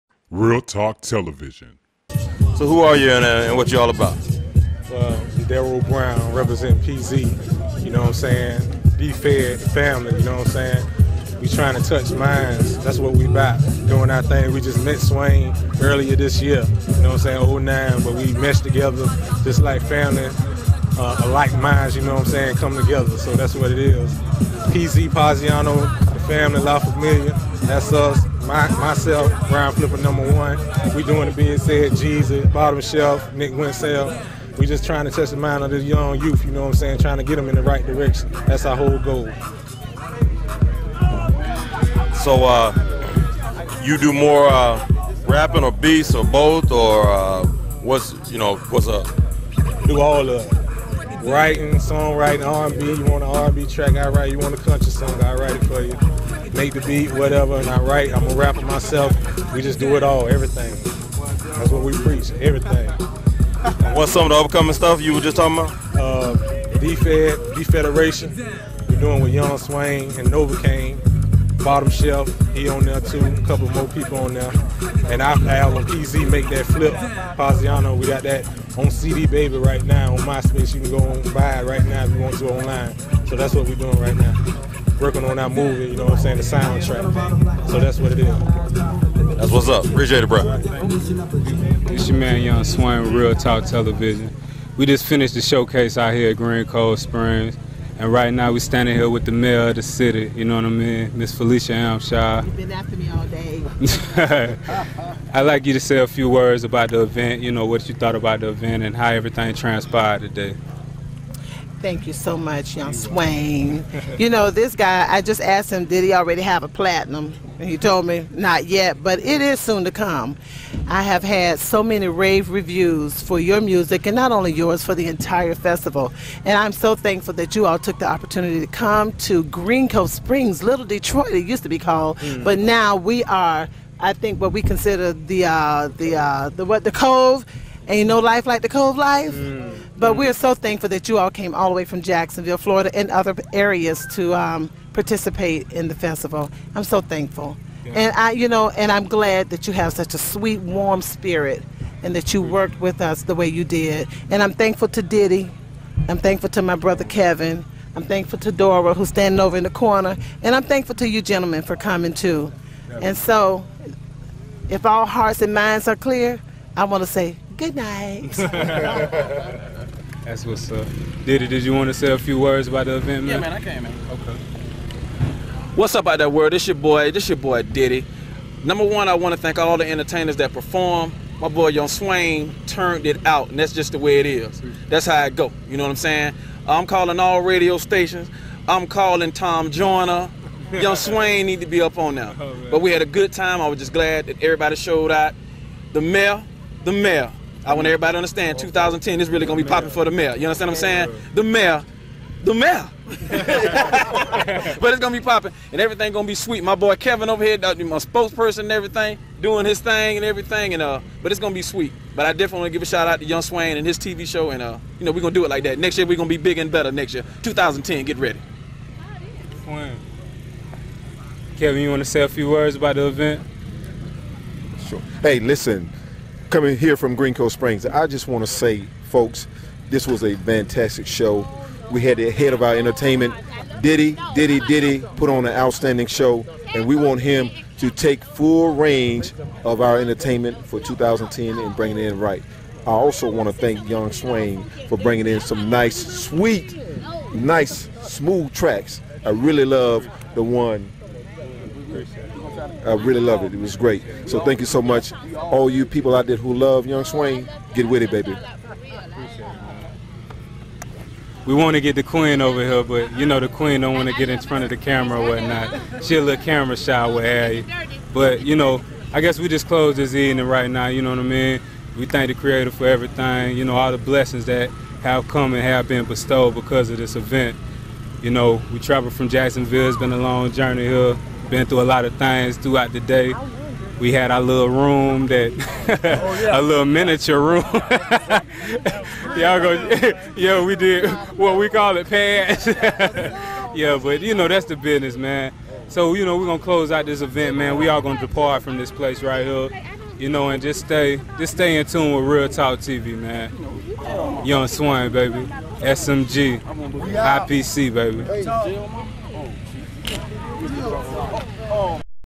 Green Cove Closure
The Closure Of The 8th Annual SoulFood Festival With A Interview With The Mayor Of Green Cove Springs